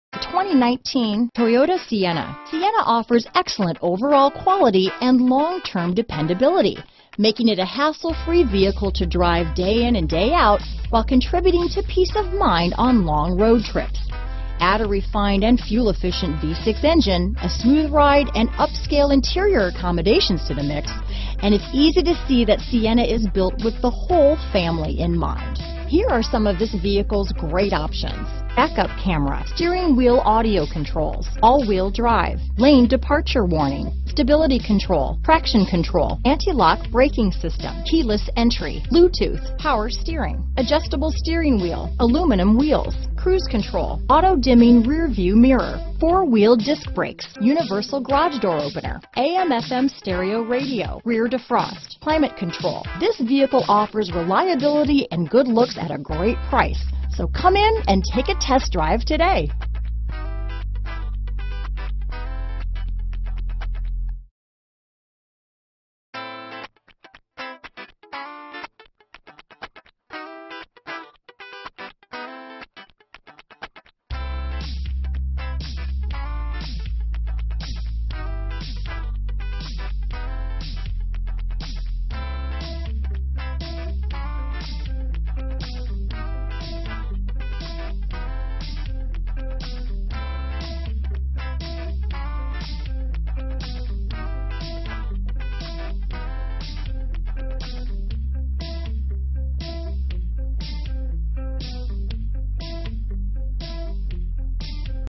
2019 Toyota Sienna Carson City, Reno, Northern Nevada, Dayton, Lake Tahoe, NV 61815
Super White New 2019 Toyota Sienna available in Carson City, Nevada at Carson City Toyota. Servicing the Reno, Northern Nevada,  Dayton, Lake Tahoe, NV area.

The 2019 Toyota Sienna is the perfect choice for families and fun. Youll always look good on the go with the unmistakable upper and lower grille openings and expanded fog light housing and 19-inch, 6-spoke gunmetal-finish alloy wheels. A new take on Swagger Wagon, the Siennas spacious dash is loaded with tech, has a new modern design and comes complete with high-end accents. The color screen display and driver-centric console make driving a breeze, literally, with three separate climate control zones. Theres never a bad seat in the house with an available dual-moonroof, and available dual power sliding side doors make it easy for the whole family to get in and out than ever before with remote keyless entry or available smart key fob. In one motion, the third-row folds into a cargo-storage area, leaving you with a flat space.